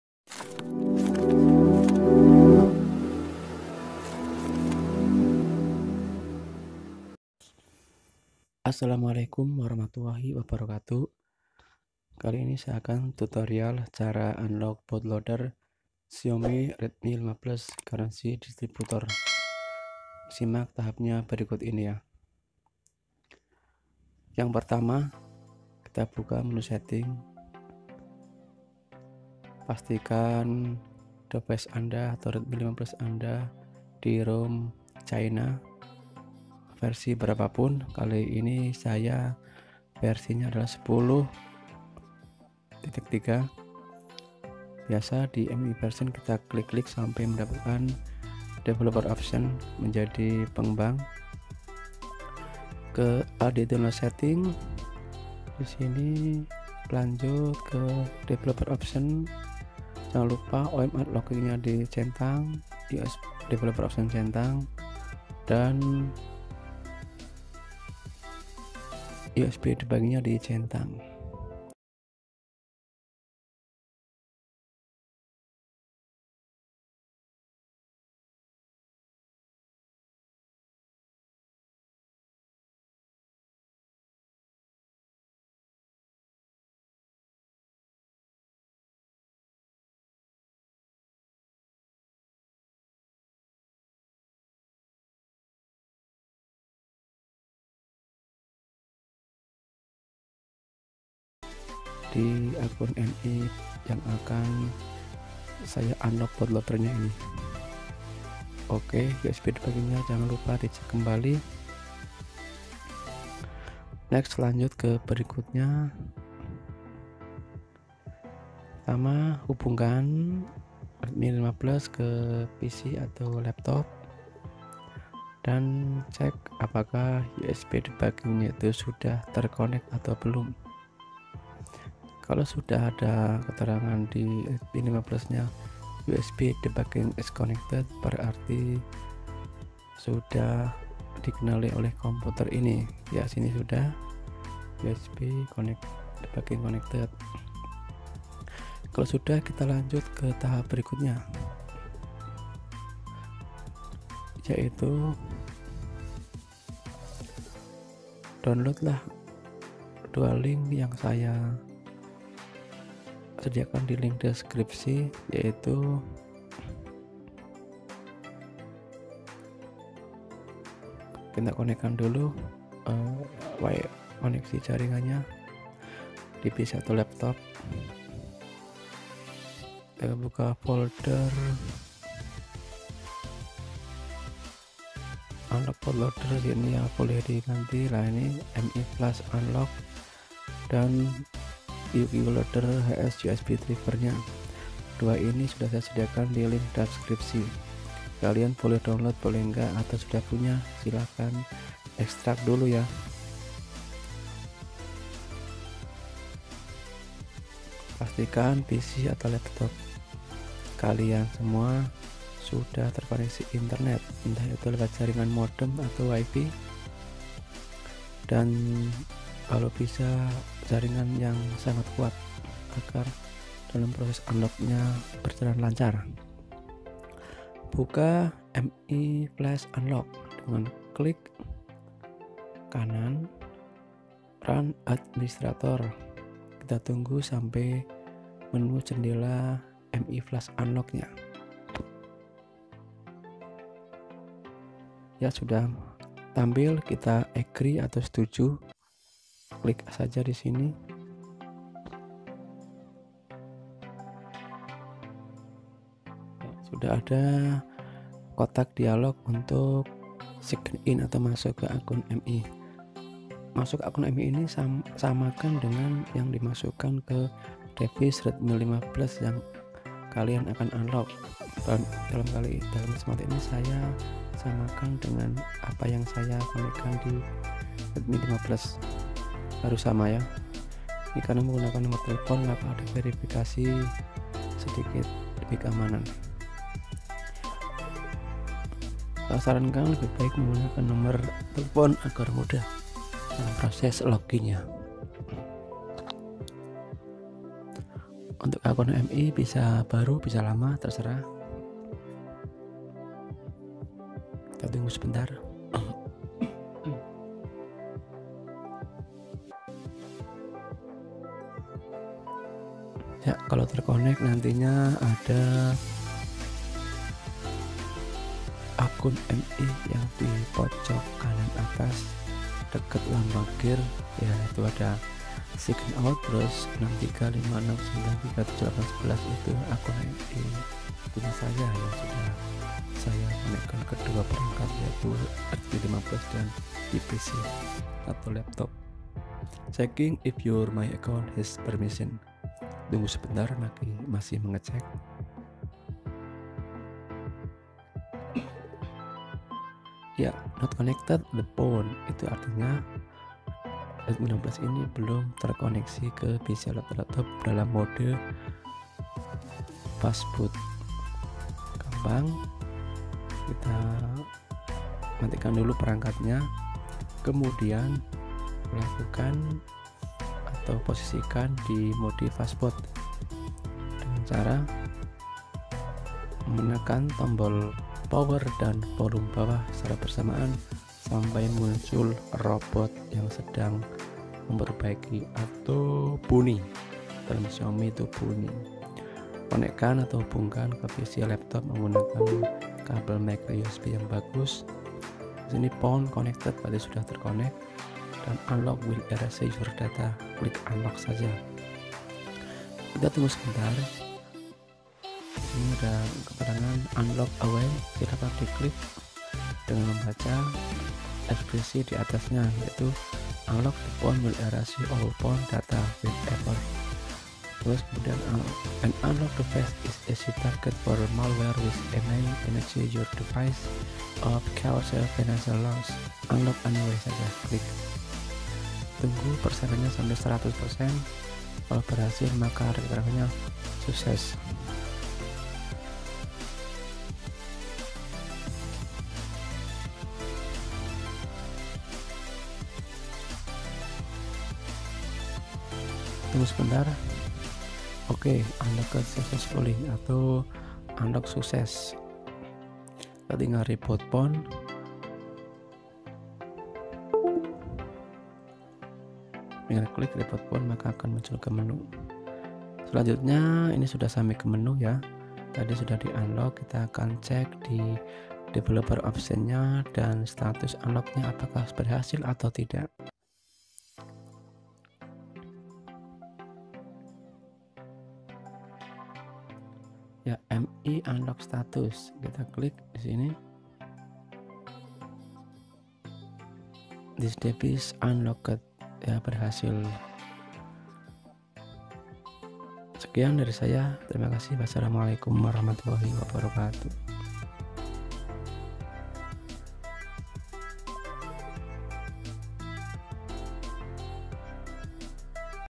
Cara Unlock Bootloader UBL Xiaomi Redmi 5 plus Distributor
Cara mudah buka kunci Bootloader Xiaomi redmi 5 plus garansi distributor

Bahan yg harus di persiapkan adalah
File mi flash unlock
Bisa di download di google atau di sini

Ikuti tahap sampai selesai di video ini.
Ada pertanyaan silahkan komen di kolom komentar

Terimakasih